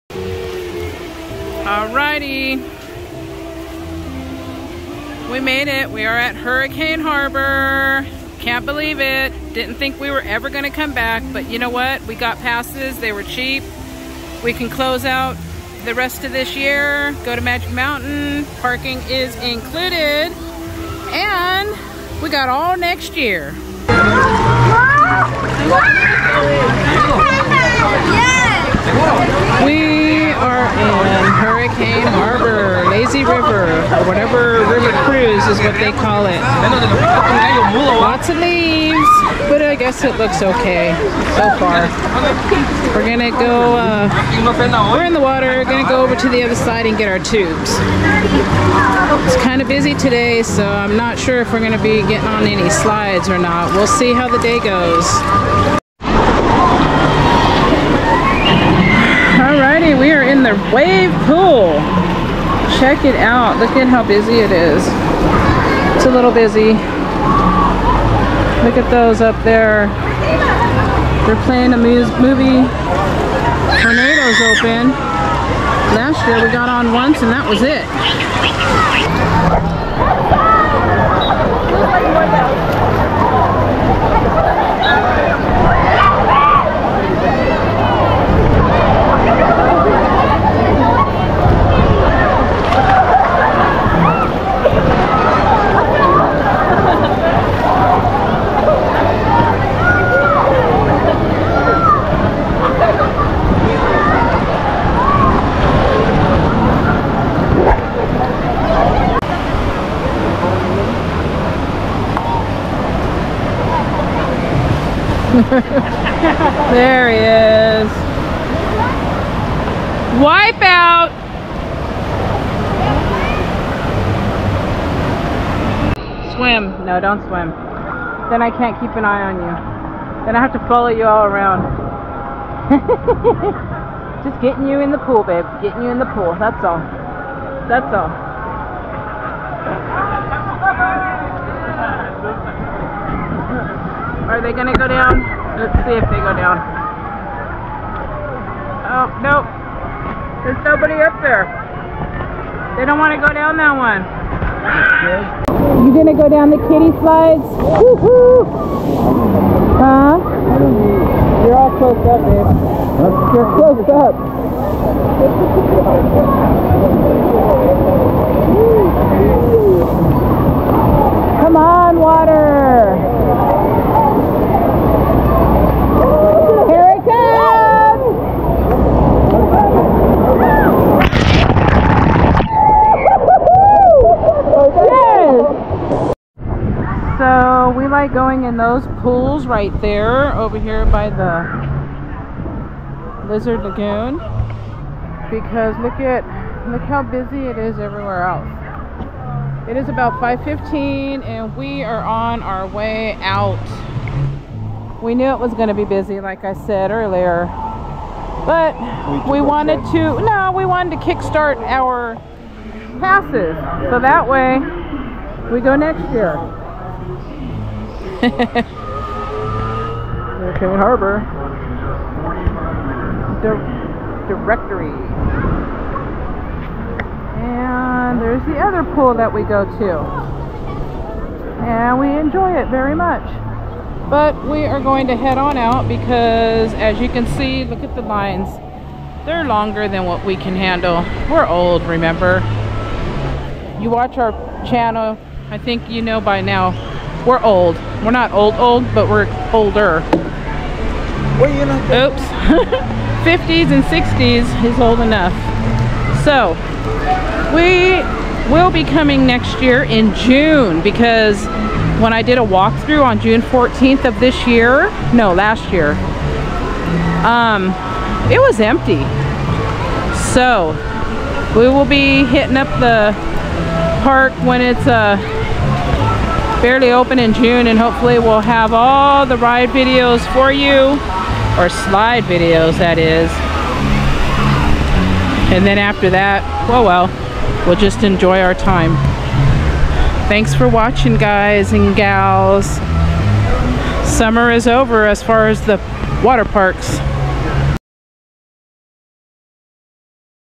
Checking out HURRICANE HARBOR!!!
Ended up getting passes for HURRICANE HARBOR 2025 & Magic Mountain. We get the rest of 2024 & was able to get a trip in. I knew they'd be busy being they're not open on the weekdays anymore. It was ok, we always make the best out of it. I know WHEN to enjoy the park to ourselves when they open next summer. Can't wait.